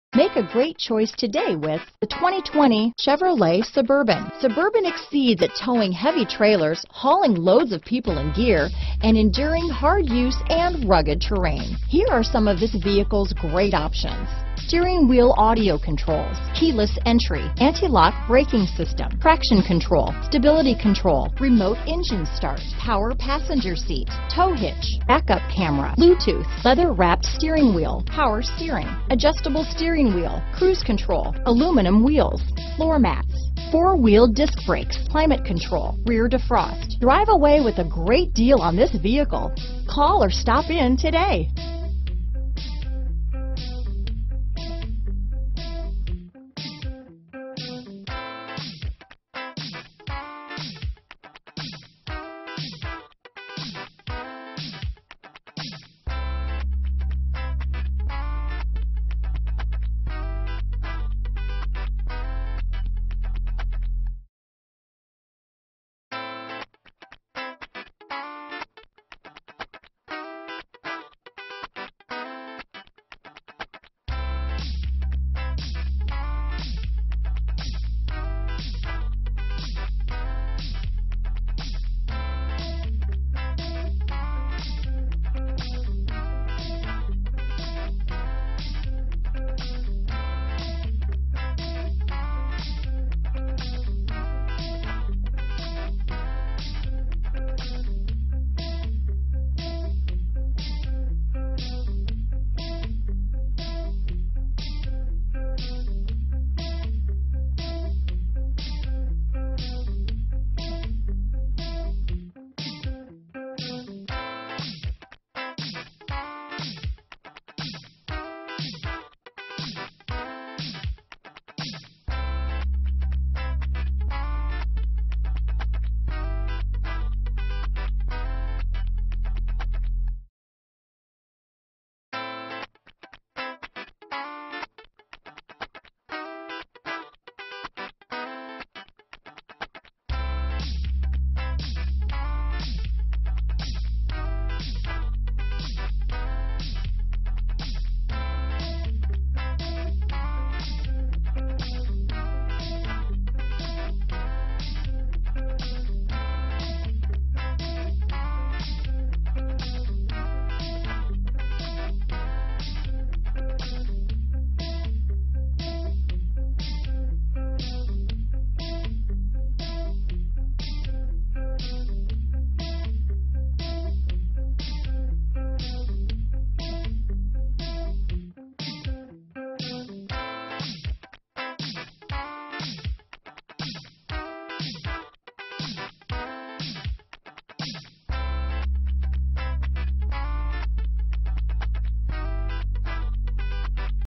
2020 Chevrolet Suburban Anaheim, Buena Park, Santa Ana, Norwalk, Fullerton, CA LR141618
Black New 2020 Chevrolet Suburban available in Buena Park, California at Premier Chevrolet of Buena Park. Servicing the Anaheim, Santa Ana, Norwalk, Fullerton, CA area.

Black 2020 Chevrolet Suburban LS RWD 6-Speed Automatic EcoTec3 5.3L V8